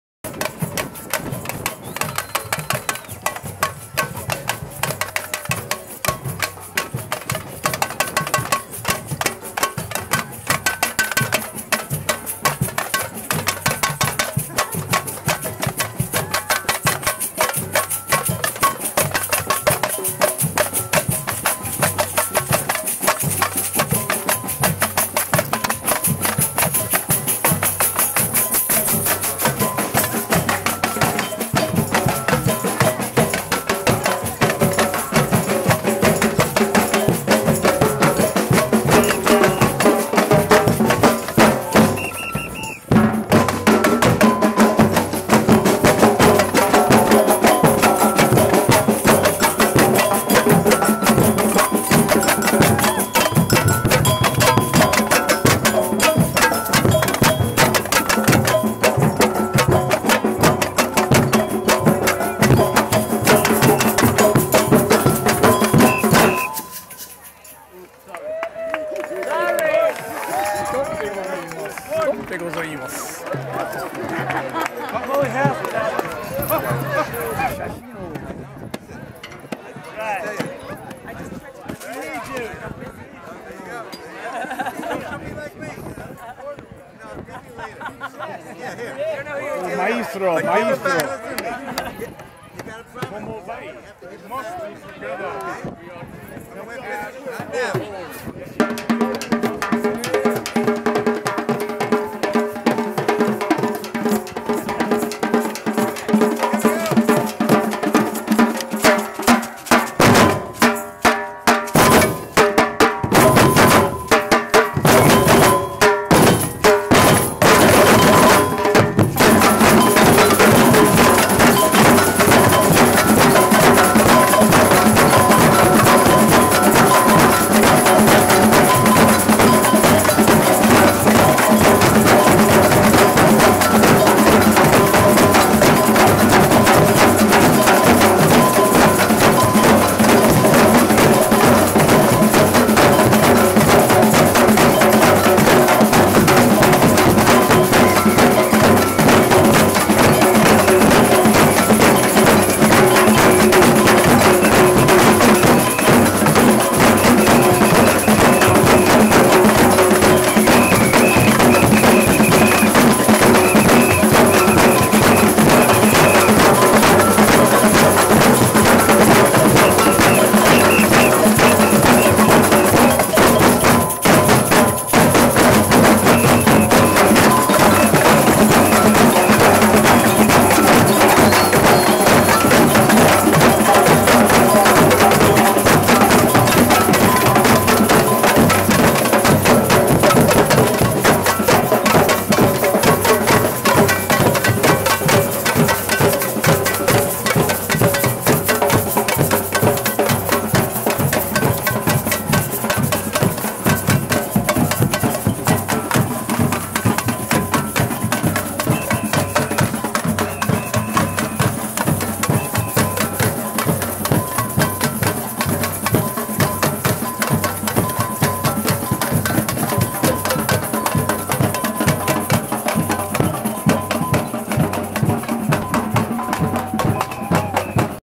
Escola de Samba Boom & TheMermaidChef August 19th, 2014/3
Welcoming the band back at where they started, and they played again!